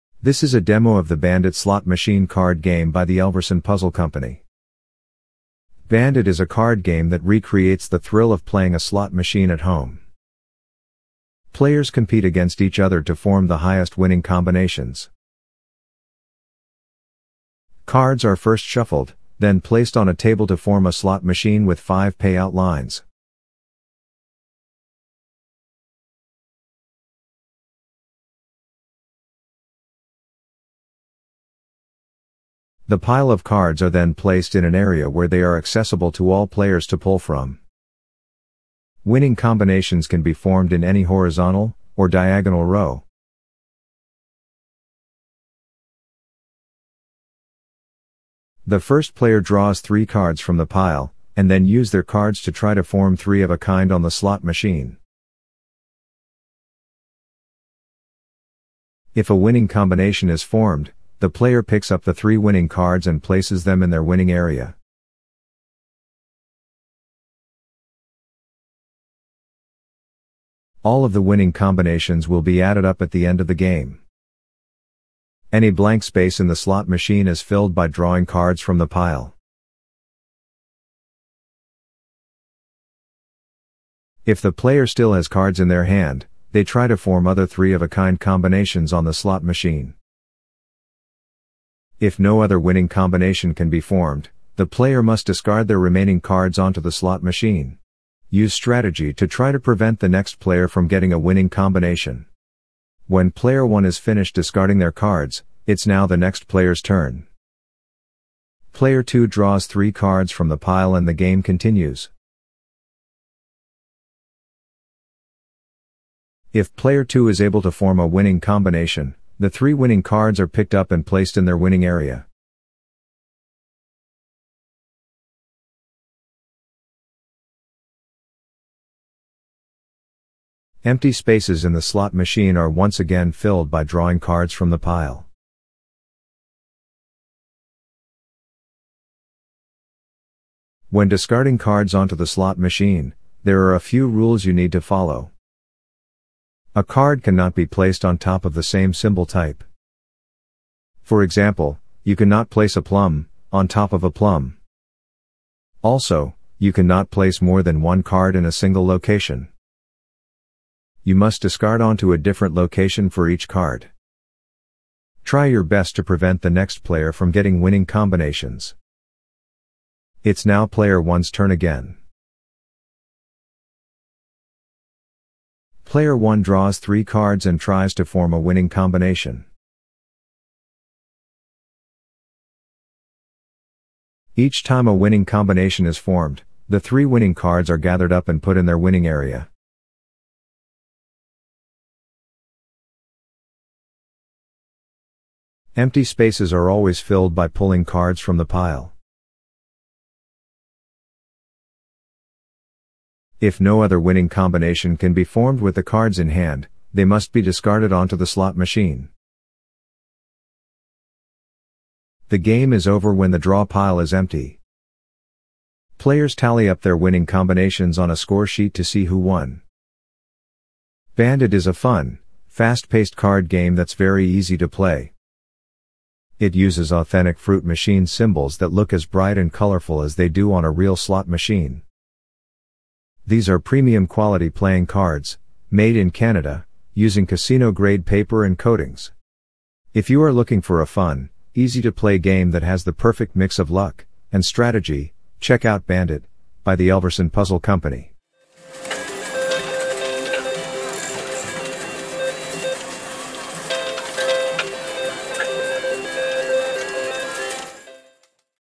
Bandit Slot Machine Card Game
This video tutorial will teach you how to play the slot machine card game Bandit.

Bandit is a family card game that recreates the thrill of playing a slot machine at home. Players compete against each other to form the highest winning combinations.

Cards are first shuffled, then placed on a table to form a slot machine with 5 payout lines. The pile of cards are then placed in an area where they are accessible to all players to pull from. Winning combinations can be formed in any horizontal, or diagonal row.

The first player draws 3 cards from the pile, and then use their cards to try to form 3 of a kind on the slot machine.  If a winning combination is formed, the player picks up the 3 winning cards and places them in their winning area. All of the winning combinations will be added up at the end of the game.

Any blank space in the slot machine is filled by drawing cards from the pile.  If the player still has cards in their hand, they try to form other 3 of a kind combinations on the slot machine.

If no other winning combination can be formed, the player must discard their remaining cards onto the slot machine.  Use strategy to try to prevent the next player from getting a winning combination. When player one is finished discarding their cards, it's now the next player's turn.

Player two draws 3 cards from the pile and the game continues. If player two is able to form a winning combination, the 3 winning cards are picked up and placed in their winning area.  Empty spaces in the slot machine are once again filled by drawing cards from the pile.

The game is over when the draw pile is empty.  Players tally up their winning combinations on a score sheet to see who won.

Bandit is a fun, fast paced card game that's very easy to play.  It uses Authentic fruit Machine Symbols that look as bright and colorful as they do on a real Slot Machine. These are Premium Quality Playing Cards, Made in Canada, using Casino Grade paper and coatings.

If you are looking for a fun, easy to play game that has the perfect mix of luck, and strategy, check out Bandit, by the Elverson Puzzle Company.

Link to Bandit Slot Machine Card Game at Elverson Puzzle Co.